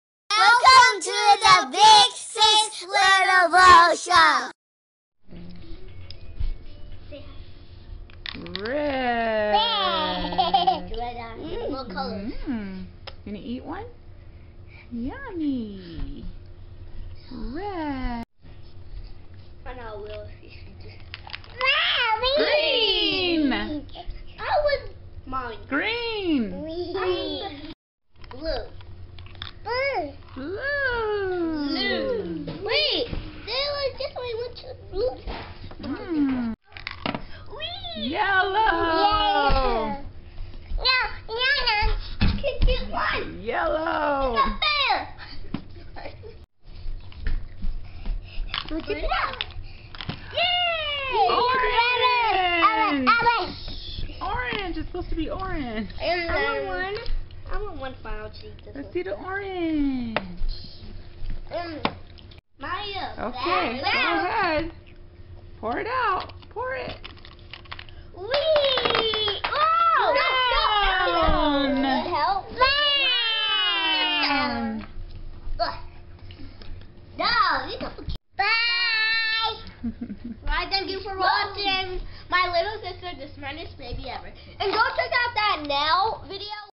Learn Colors with M&M's Candy
Learn Colors with M&M's candy. Lil Sis learning colors with big sis and lil bro eating M&M's. Kids channel. Opening new toys, playing, making skits, challenges, and having fun.